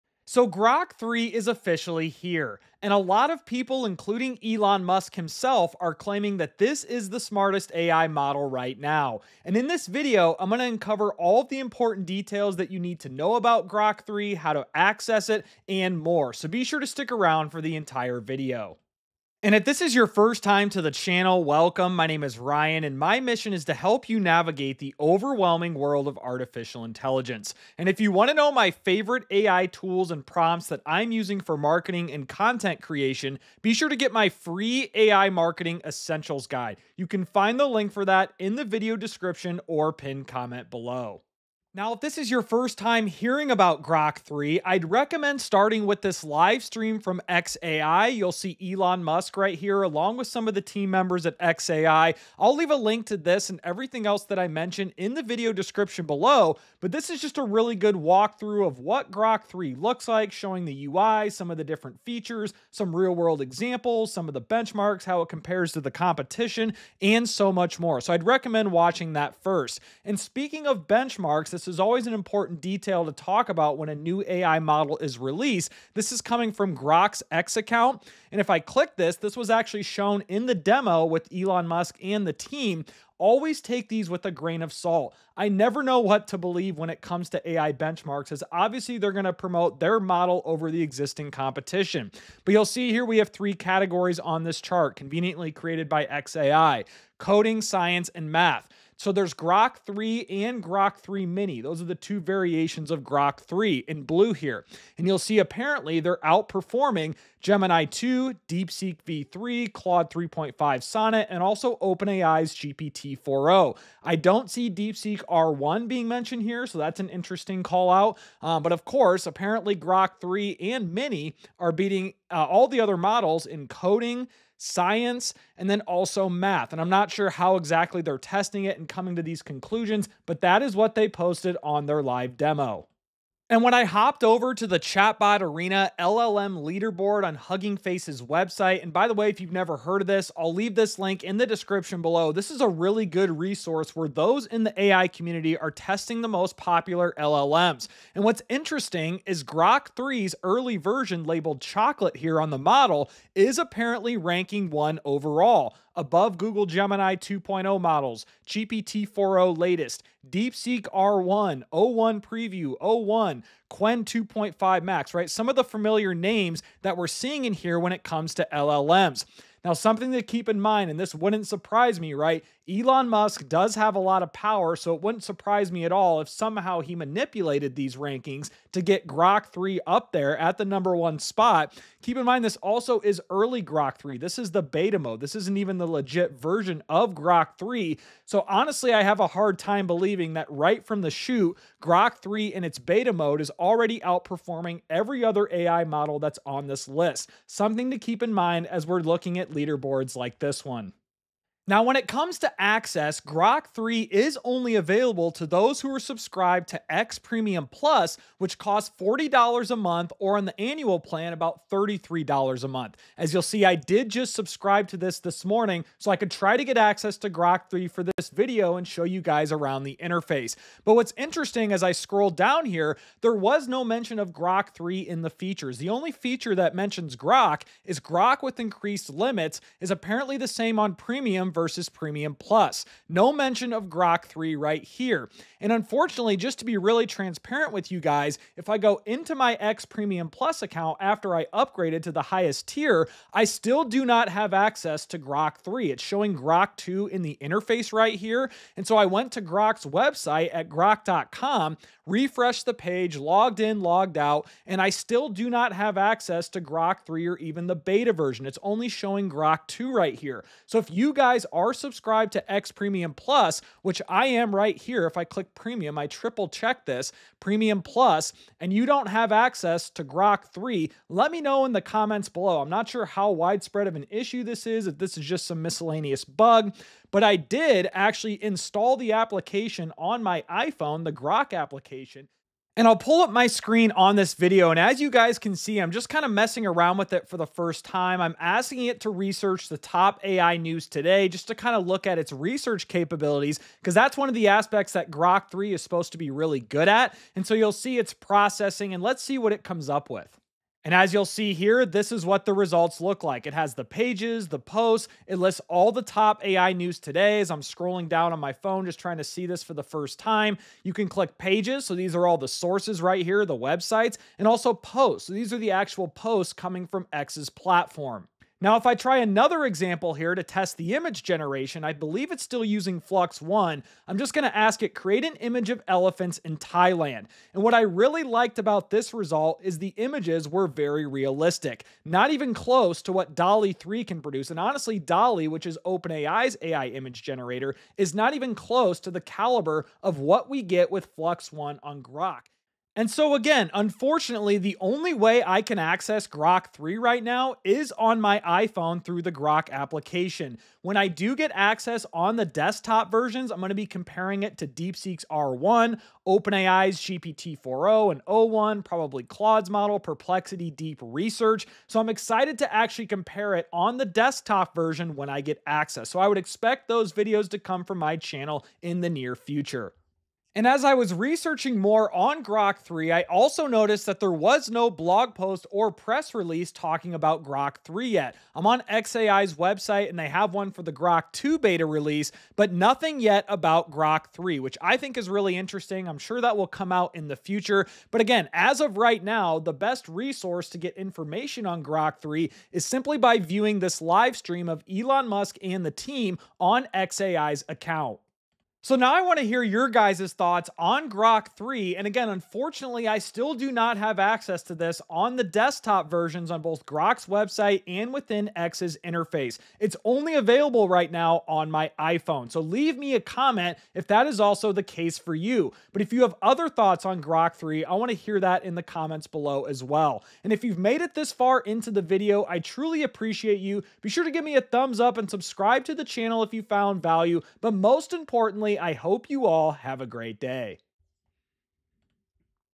Grok 3 is Here! Is Elon Musk's New AI Model the Best?
Grok 3 has arrived, and this video dives into Elon Musk's latest AI model. Does Grok 3 live up to the hype?

Time Stamps:
0:00 Intro
0:42 Grok 3 Details
2:13 LLM Leaderboard Rankings
3:21 Grok 3 Access
4:42 Grok iOS App
6:10 Final Thoughts